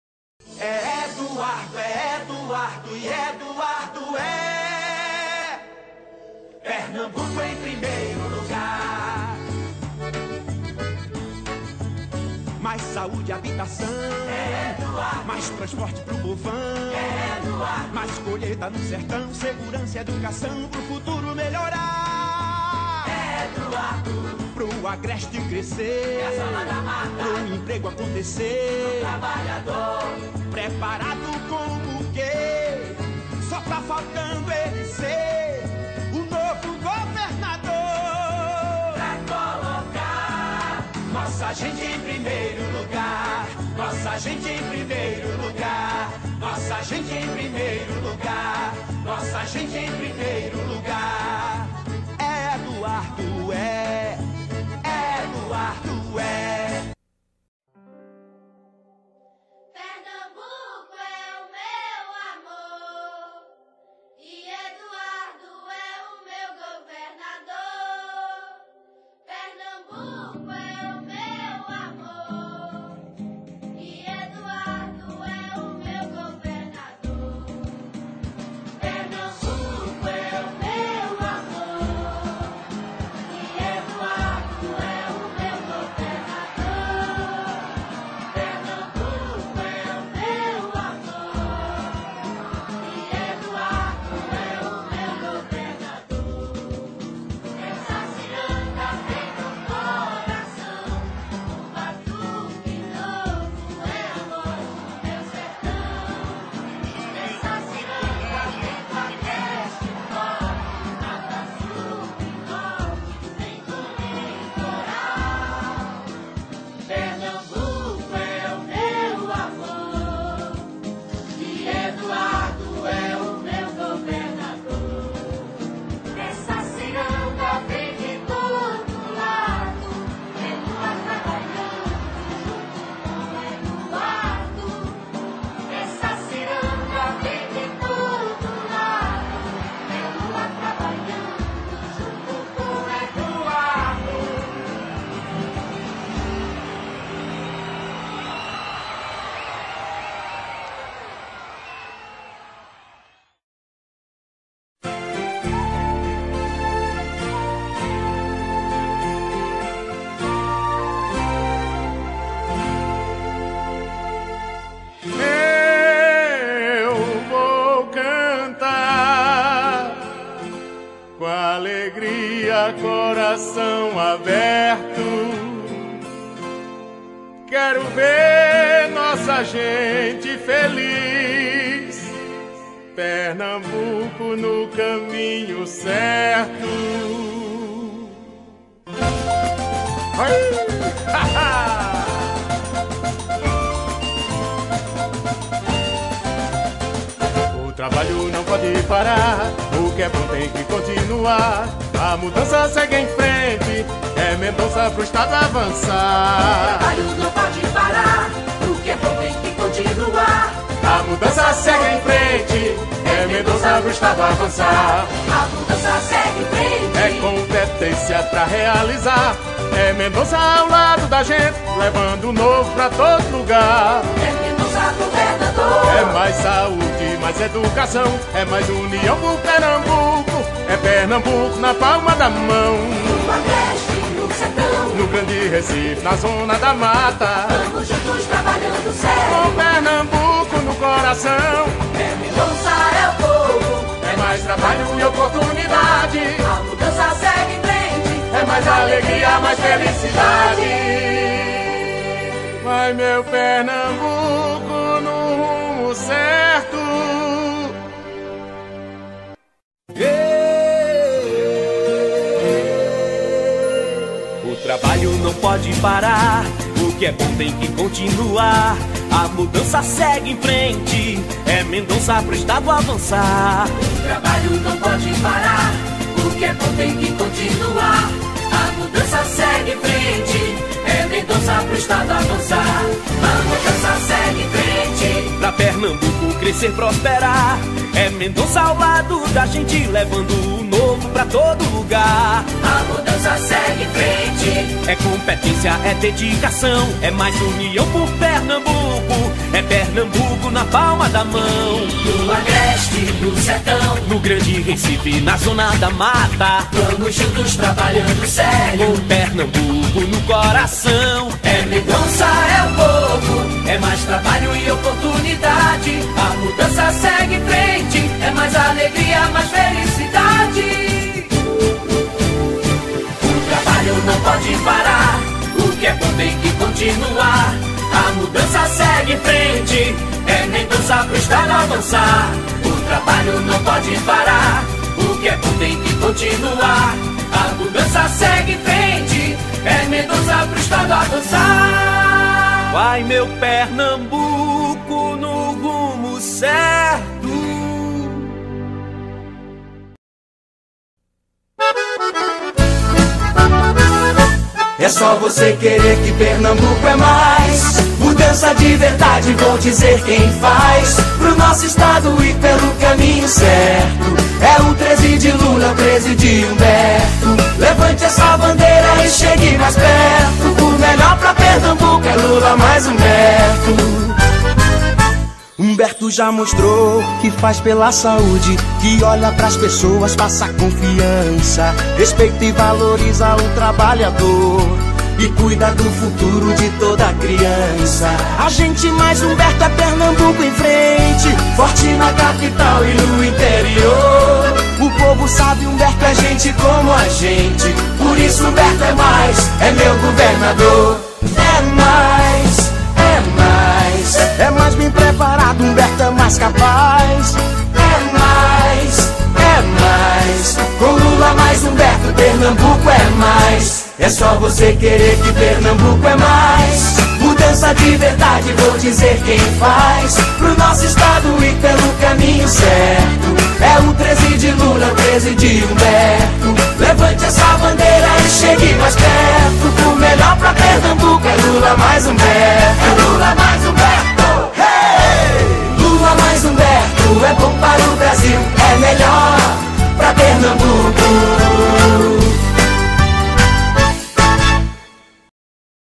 Jingles Pernambuco 2006 | Governador
Jingles:
00:00 - Eduardo Campos (PSB)
02:34 - Mendonça (PFL)
05:51 - Humberto (PT)